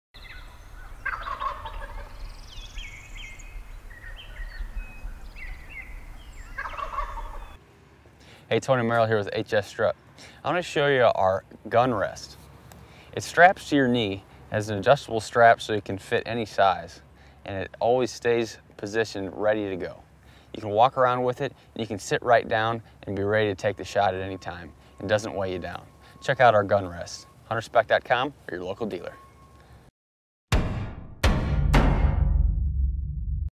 Hunters Specialties Gun Rest Video
When your on the move and you need to sit down and get ready for a quick shot, you need a gun rest that is instantly at the ready.  Our Gun Rest never leaves your knee and is ready for action as soon as you sit down.  Watch as Hunters Specialties' own Tony Mehrl shows how it's used to get you ready for turkey season this year (or really any other season too for that matter!)